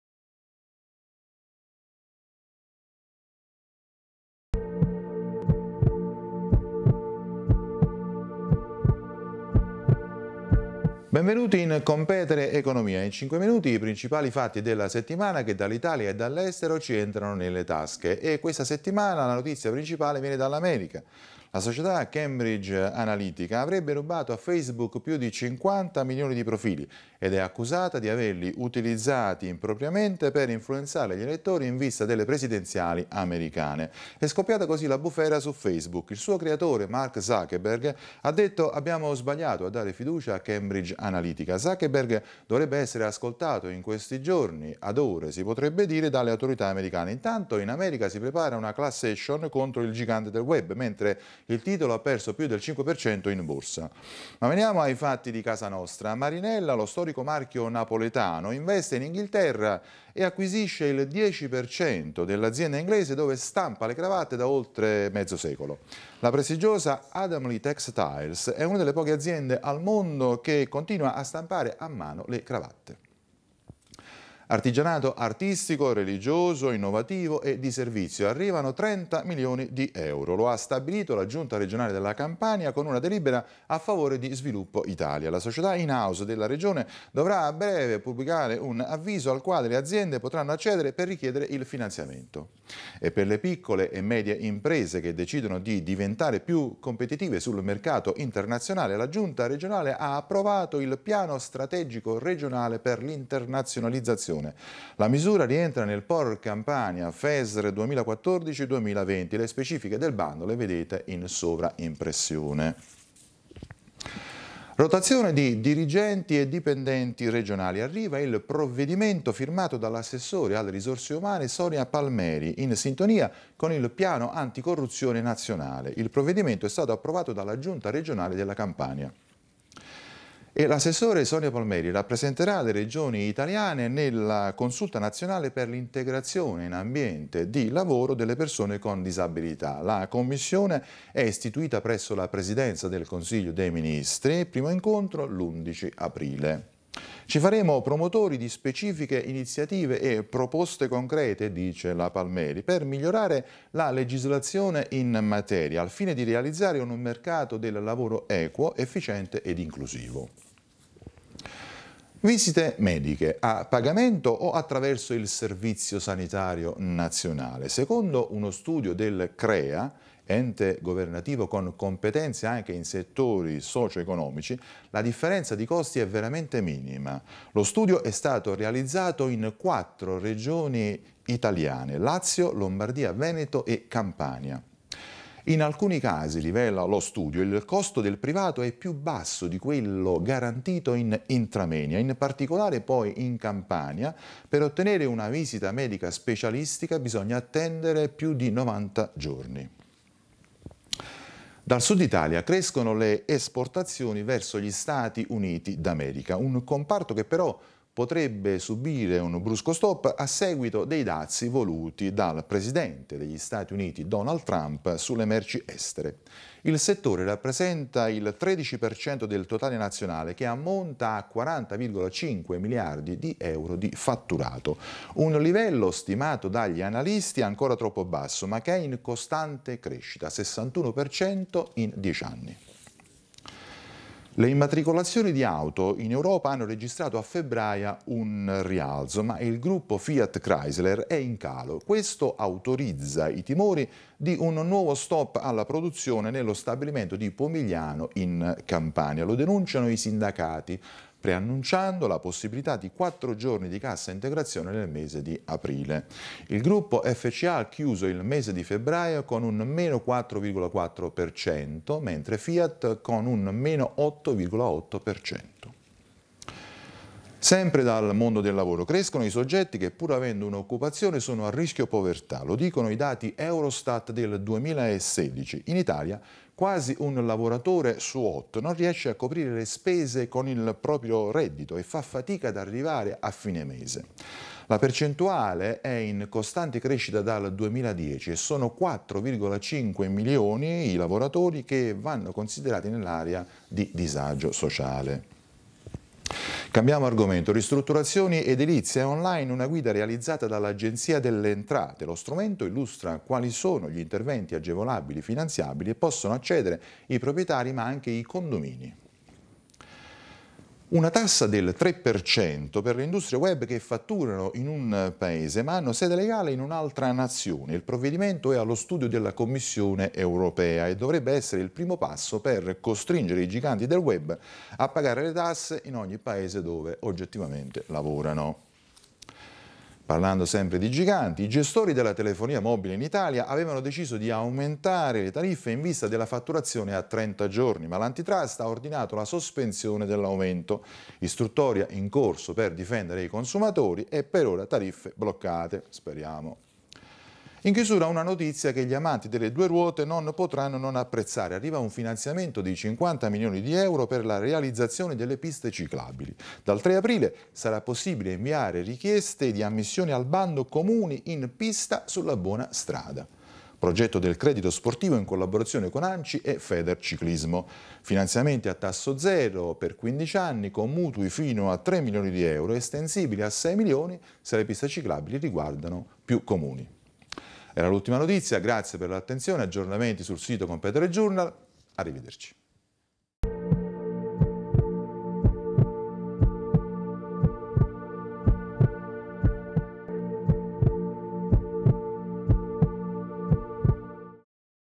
Kompetere News Economia del 23 Marzo 2018
Kompetere Journal, rivista online innovativa di profilo economico, propone ai suoi lettori un settimanale di news che guarda attentamente ai fatti nazionali e del Mezzogiorno italiano.
Ogni venerdì, alle ore 14:00, su tutti i canali social di Kompetere Journal.
Conduce il giornalista Giuseppe de Silva
Direttore Massimiliano Musto
Responsabile Redazione Nicole Lanzano
Riprese e Montaggio Michele Vitale e Tony Ariosto